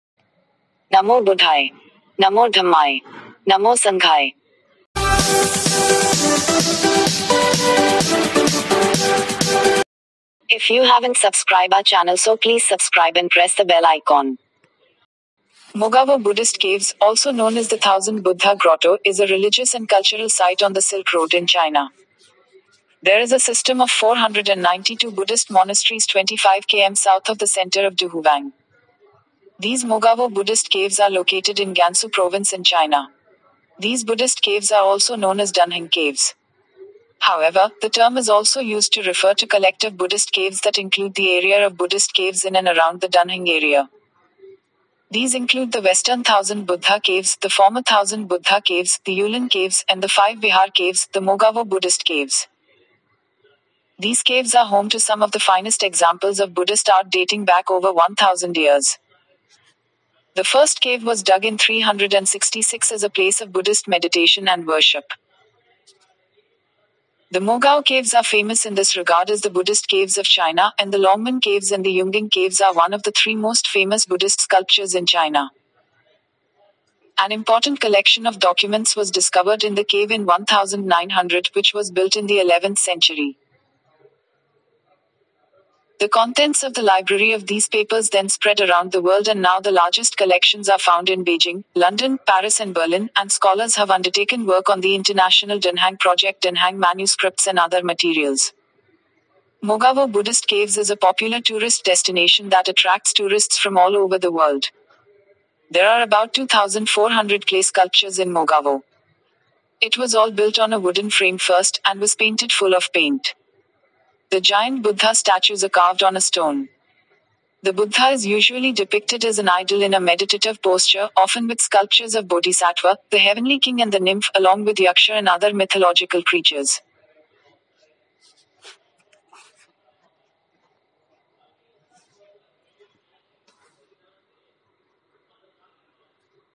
The Mogao caves || Thousand Buddha Grottoes or Caves of the Thousand Buddhas
The Mogao caves
Thousand Buddha Grottoes or Caves of the Thousand Buddhas
Buddh grottoes
Lord Buddha Tathagat Buddha
Siddharth Gautam Buddha

Mogavo Buddhist Caves, also known as the Thousand Buddha Grotto, is a religious and cultural site on the Silk Road in China.  There is a system of 492 Buddhist monasteries 25 km south of the center of Duhuang.  These Mogavo Buddhist caves are located in Gansu Province in China.  These Buddhist caves are also known as Dunhung Caves.  However, the term is also used to refer to collective Buddhist caves that include the area of ​​Buddhist caves in and around the Dunhung area.
 These include the Western Thousand Buddha Caves, the former Thousand Buddha Caves, the Yulin Caves, and the five Vihar Caves, the Mogavo Buddhist Caves.  These caves are home to some of the finest examples of Buddhist art dating back over 1000 years.  The first cave was dug in 366 as a place of Buddhist meditation and worship.  The Mogao Caves are famous in this regard as the Buddhist Caves of China, and the Longmen Caves and the Yungang Caves are one of the three most famous Buddhist sculptures in China.

 An important collection of documents was discovered in the cave in 1900, which was built in the 11th century.  The contents of the library of these papers then spread around the world and now the largest collections are found in Beijing, London, Paris and Berlin, and scholars have undertaken work on the international Dunhang project Dunhang manuscripts and other materials.
 Mogavo Buddhist Caves is a popular tourist destination that attracts tourists from all over the world.  There are about 2,400 clay sculptures in Mogavo.  It was all built on a wooden frame first, and was painted full of paint.  The giant Buddha statues are carved on a stone.  The Buddha is usually depicted as an idol in a meditative posture, often with sculptures of Bodhisattva, the heavenly king and the nymph, along with Yaksha and other mythological creatures.